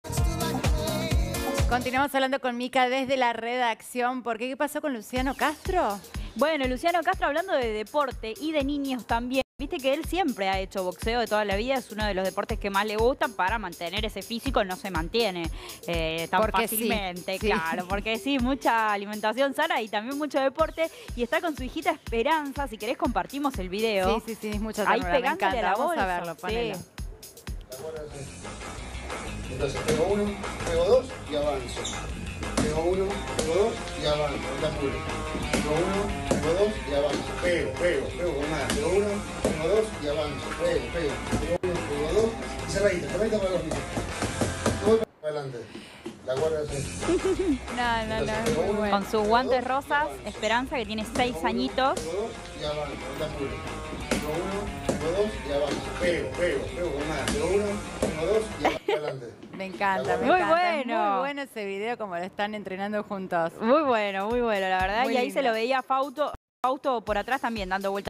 El video de Luciano Castro entrenando con su hijita Esperanza: "Pego uno, pego dos, ¡y avanzo!"
Los que lo conocen lo saben: Luciano Castro es un apasionado del boxeo. Además de practicar, viaja a Las Vegas cuando sus amigos boxeadores tienen una pelea importante y está al tanto de las últimas novedades de este deporte que tanto ama.

Y ahora su hija, Esperanza (6), fruto de su relación con Sabrina Rojas, también se copó con el box. Contento, el actor compartió a través de sus historias de Instagram un video entrenando con su pequeña... ¡reunidos por la misma pasión!

En el clip, se puede ver a Esperanza dando sus primeros pasos como mini boxeadora y luciendo unos guantes rosas, mientas su papá la alienta: "Pego uno, pego dos, ¡avanzo!". Mientras tanto, la niña rie a carcajadas al ver a su padre tan entusiasmado.